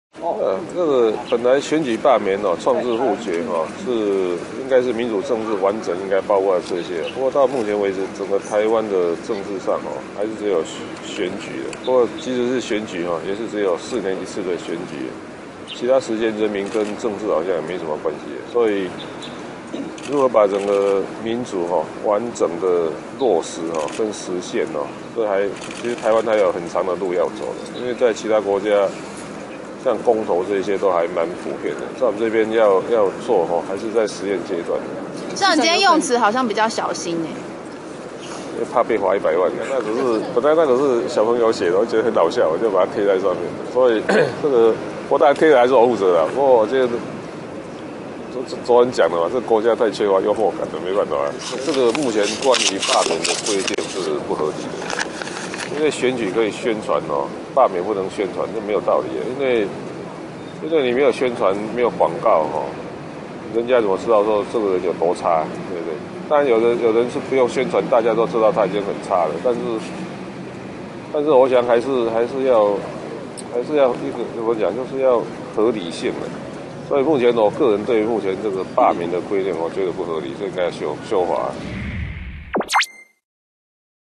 視察蔡正元罷免投票 ​柯P：罷免規定應修法
視察蔡正元罷免投票 ​柯P：罷免規定應修法2015年02月14日國民黨立委蔡正元罷免投票今天舉行，台北市長柯文長上午8時20分在副市長鄧家基與民政局長藍世聰陪同下，前往西湖國中投票所視察，了解選務工作安排與投票情形。柯文哲視察結束受訪表示，目前關於罷免規定不合理，選舉可以宣傳，罷免不能宣傳，沒有宣傳廣告人家怎知道這人有多差，有人是不用宣傳大家都知道很差，規定還是要合理性，「我對目前罷免規定不合理應該要修法。」

記者上午走訪西湖國中與德明科大投票所，可能是因為才剛開始投票，人潮並不多，稀稀疏疏。柯文哲表示，這是他本人上任以後，第一次台北市有舉辦選舉事務，因此現場看看選務工作，對基層選務工作人員表達慰問。民主政治成本滿昂貴的，一個選舉事務所要9人，今天開了130多個投開票所，動員上千人，這些都是成本。

有媒體詢問之後有沒有考慮使用iVoting罷免市府官員？柯說，選舉、罷免、創制、複決是民主政治應該包括的事情，目前台灣還是只有4年一次的選舉，其他時間人民跟政治好像沒有關係，如果把整個民主完整落實與實現，台灣還有很長路要走，其他國家公投還滿普遍的，台灣這邊要做還是在實驗階段。

對於臉書貼出罷免藏頭詩恐遭罰款，柯說，那是有小朋友寫，他貼上去，但貼了還是他負責，這個國家缺乏幽默感。